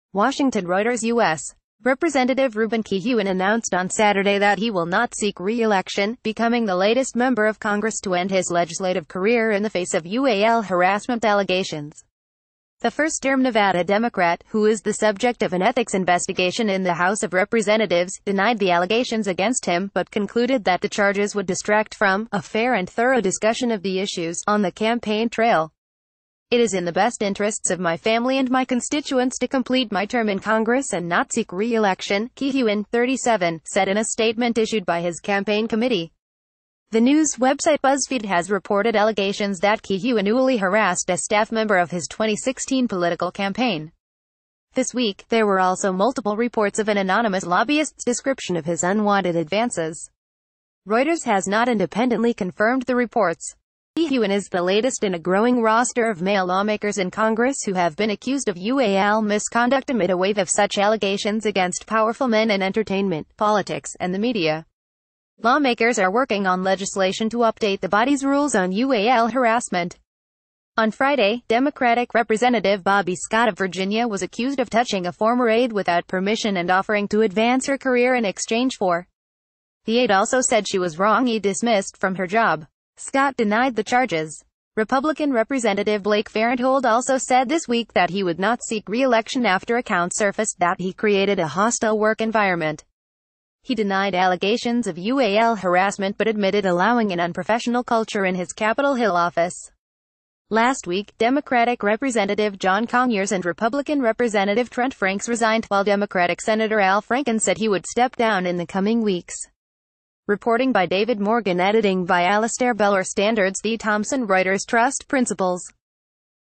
TODAY NEWS - Deputies will not seek relection in the context of the misconduct probe
Host News - TODAY NEWS - Deputies will not seek relection in the context of the misconduct probe

WASHINGTON Reuters U.S. Representative Ruben Kihuen announced on Saturday that he will not seek reelection, becoming the latest member of Congress to end his legislative career in the face of ual harassment allegations. The firstterm Nevada Democrat, who is the subject of an ethics investigation in the House of Representatives, denied the allegations against him but concluded that the charges would distract from “a fair and thorough discussion of the issues” on the campaign trail. “It is in the ...